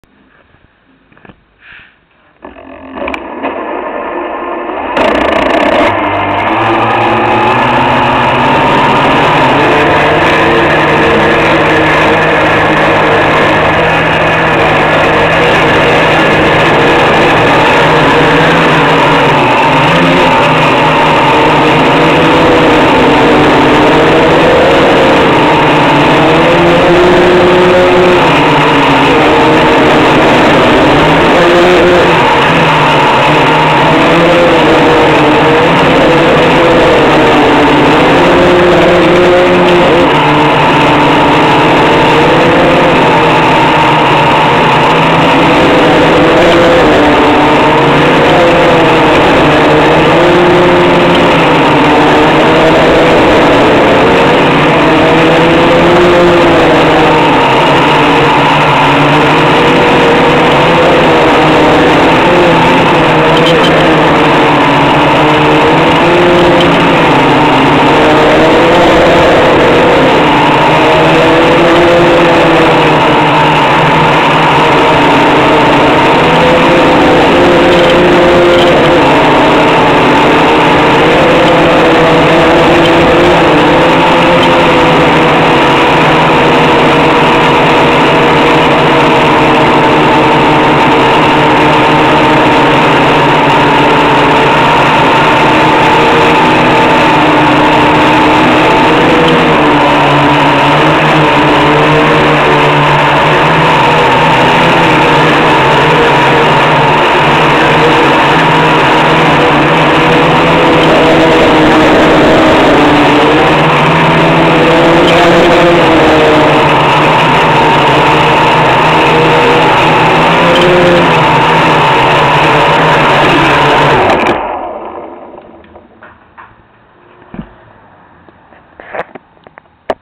Quadrocopter Test Flight onboard
First tesflight outdoor with new controller, it is much more stable